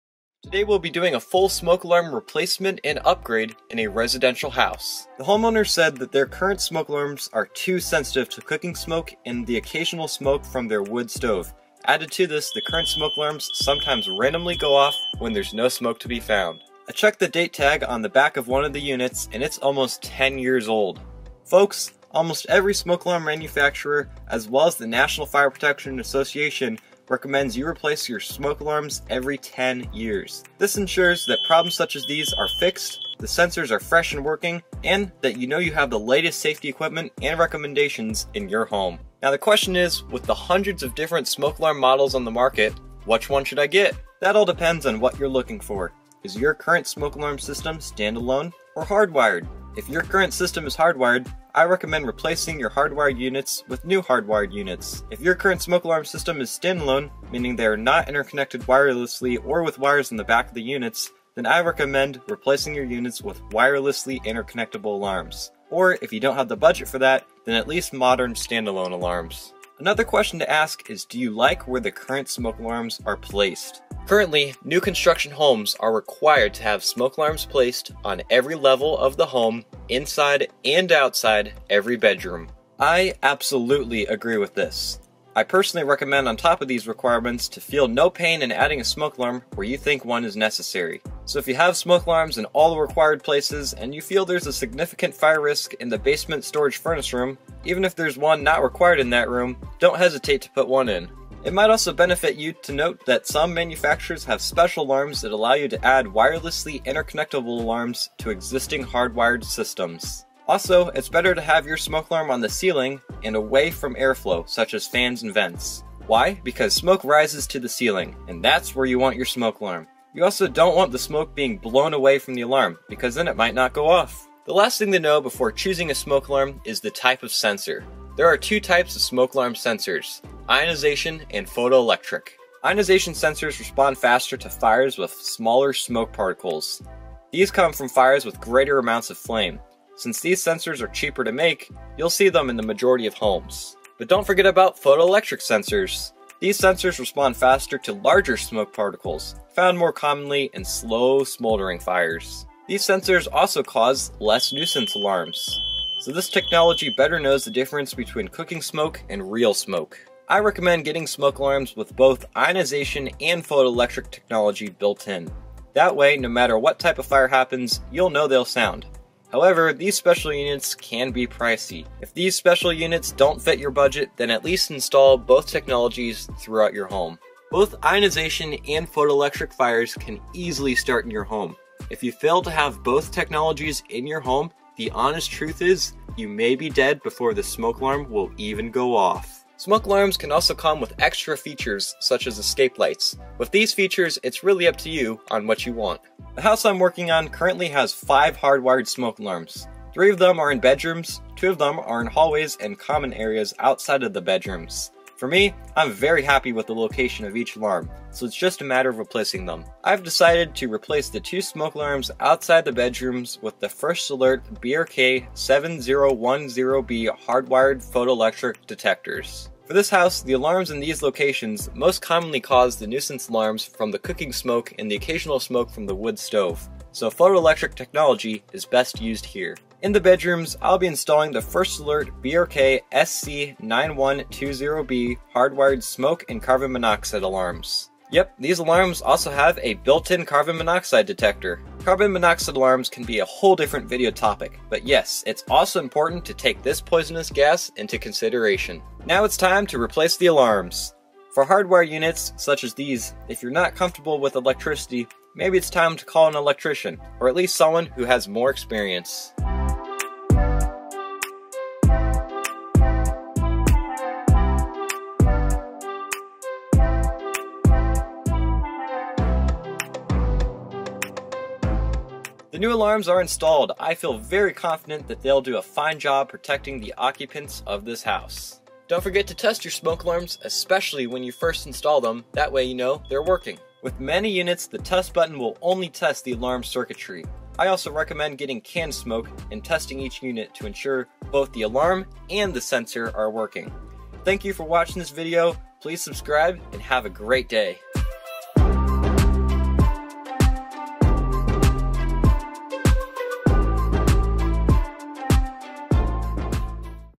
The Best Smoke Alarm for Your Home | Requirements & Fun Facts
With hundreds of different smoke alarm models on the market choosing the right one for your home is confusing! Learn the key points for choosing the right smoke alarm and its placement for your residence.

Other information to take into consideration:

Many studies have shown children are unresponsive to the standard loud high pitched smoke alarm alert tone when they're in deep stages of sleep. I've even had a similar experience when I was a child. An alarm went off in the middle of the night and my mind wasn't awake enough to register what was happening. This situation can be VERY dangerous especially when adults are unable to guide the child safely out of the house. If you have younger people in your home I recommend installing a voice alarm in and near their bedroom(s). Studies show children will respond faster if there is a voice telling them what the situation is and what to do. There are even special smoke alarm models where you can record personalized voice message so that it automatically plays when there is smoke.

I recommend getting a smoke alarm that is Underwriters' Laboratories Listed (UL). This means the unit has been put through rigorous testing to ensure it will work properly if it is maintained according to the manufacture's instructions. Most smoke and carbon monoxide alarms you see in popular stores such as Walmart or The Home Depot are UL Listed.

Lastly, getting a little nerdy, there is a difference in the term "smoke alarm" and "smoke detector". Smoke alarms are referred to in the professional industry as a device that both detects smoke and alerts to a fire with a built-in alarm. Smoke detectors are generally used on a fire/security alarm system and they simply detect smoke and transmit a signal to a different device such as a panel.

Check out SERSAFETY.COM